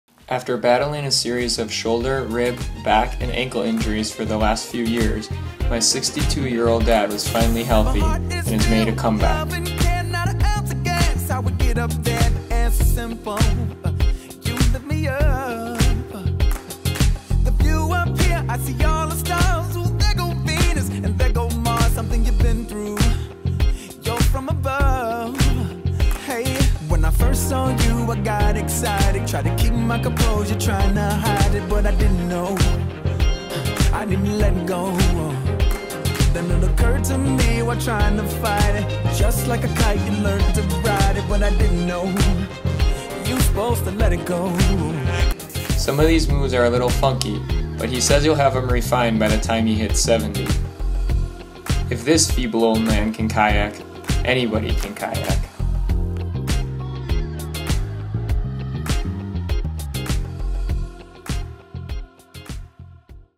2016… The comback
After battling a series of shoulder rib, back, and ankle injuries my 62 year old dad finally was healthy and made a comeback.